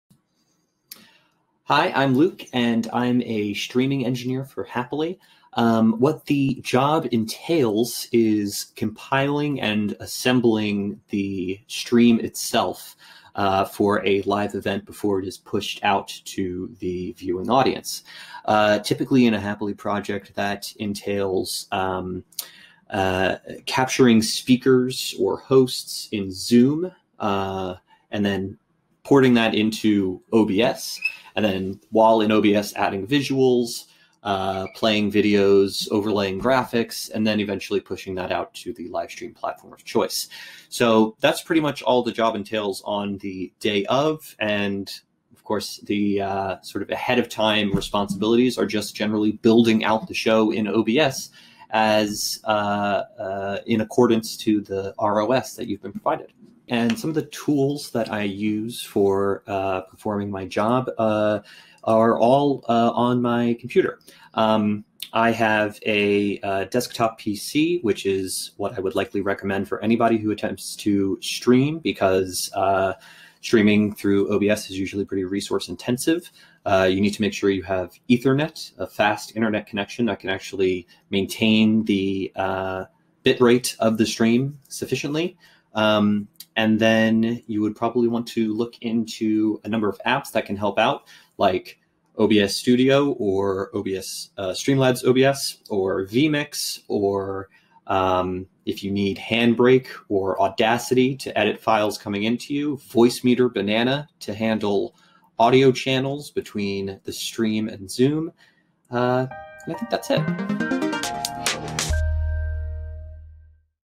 How to be a Streaming Engineer for Happily Virtual Events
Learn the basics, as well as some pro tips, about being a streaming engineer with Happily.